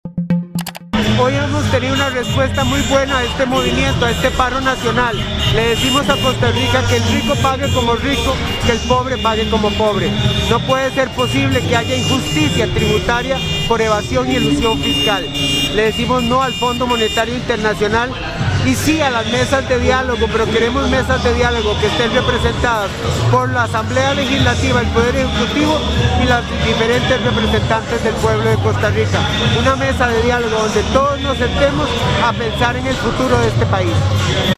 Sindicatos protestan contra planes económicos del Gobierno de Costa Rica
Sindicatos de los sectores de salud y educación de Costa Rica realizaron este lunes manifestaciones en todo el país para expresar su rechazo a planes económicos del Gobierno, principalmente nuevos impuestos, un eventual acuerdo con el Fondo Monetario Internacional y un proyecto que reforma el empleo público.
Tiempo 00:34